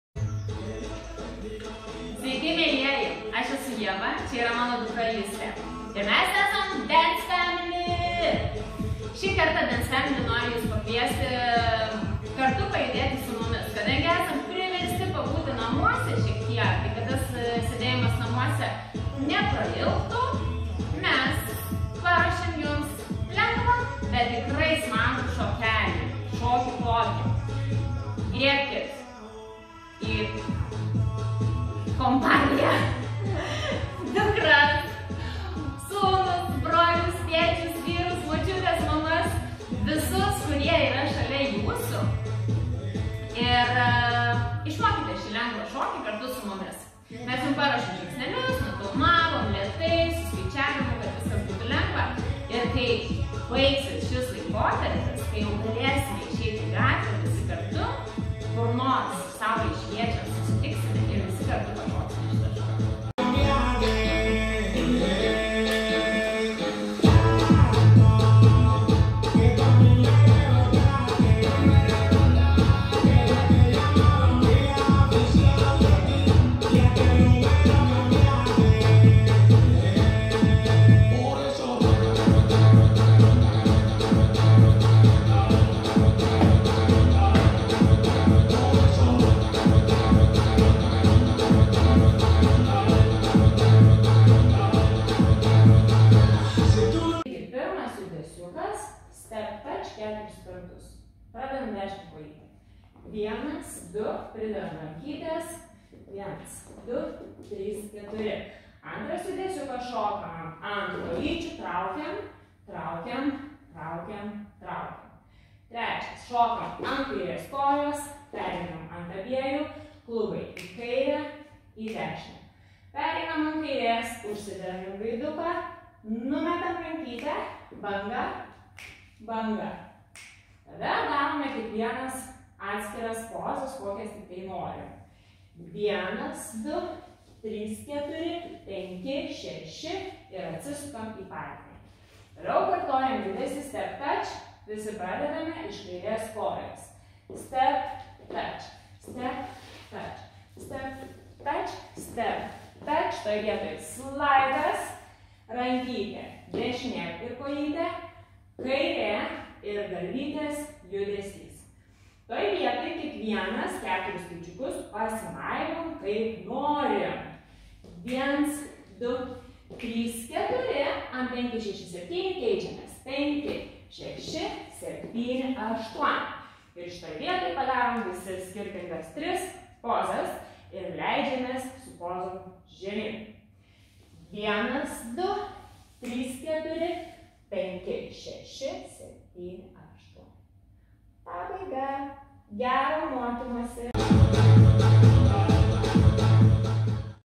Mom and daughter dance choreography / Šokantys tėvai ir vaikai, choreografija
Sveiki, esu šokių mokytoja Ieva ir mano dukra Justė. Kvečiame prisijungti prie mūsų ir išmokti linksmą choreografiją. Pasikvieskite savo mamą, tėtį, sesę, brolį, močiutę ir pajudėkite kartu su mumis :)

Mom and daughter dance choreography for all moms and daughters :)
Hi, I am a dance teacher Ieva and my daughter Justė. We invite you to join us and learn fun choreography. Invite your mom, dad, sister, brother, grandmother and dance with us :)